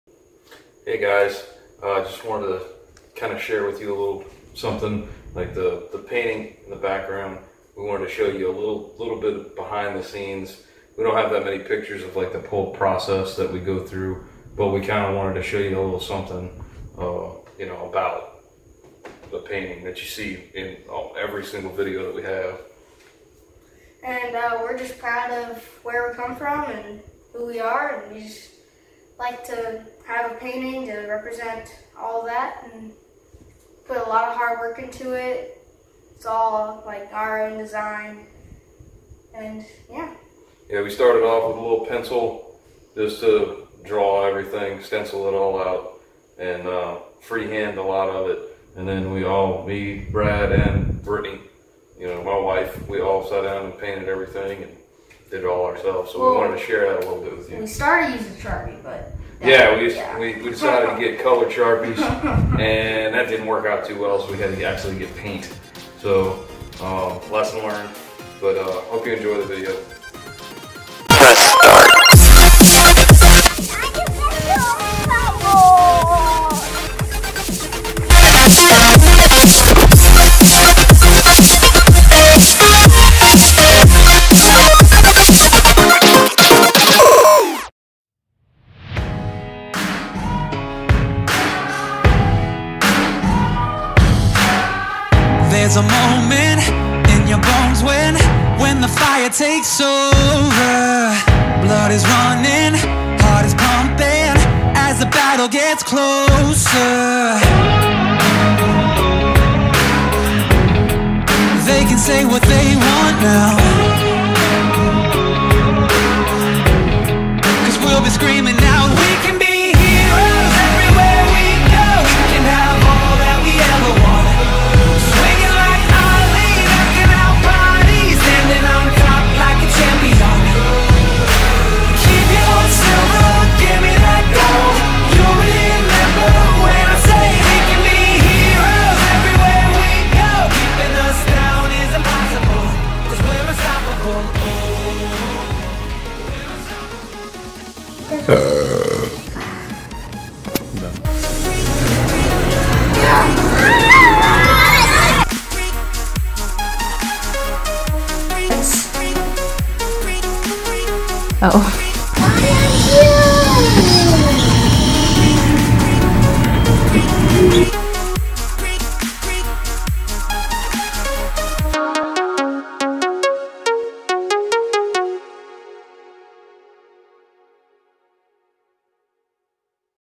Louisiana
We wanted to share with you our home made Louisiana Painting.  We cut the plywood ourselves, drew everything with pencil first, tried to use colored markers but failed miserably, then painted the entire piece.  We added the pelican after I had hung the piece on the wall, so that was challenging.  See yall in the next video.